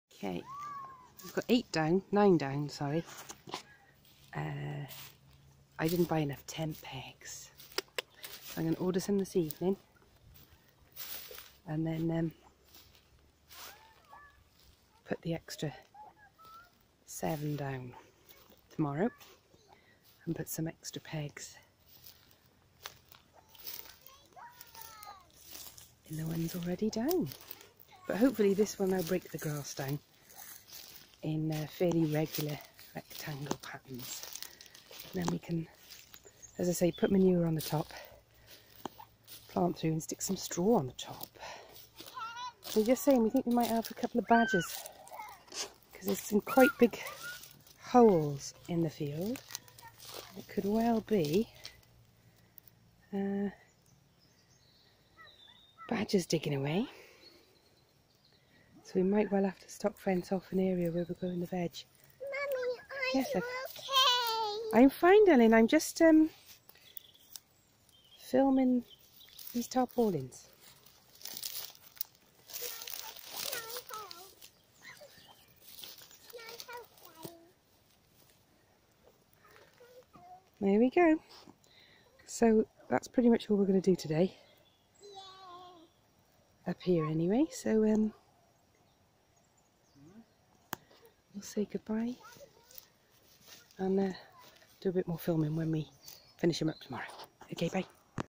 The Journey of a Mini Country Smallholding in Cornwall UK- #3 Tarpaulin Job Nearly Finished
Bad planning! Not enough tent pegs for tarpaulins for the vegetable beds, will not make this mistake twice.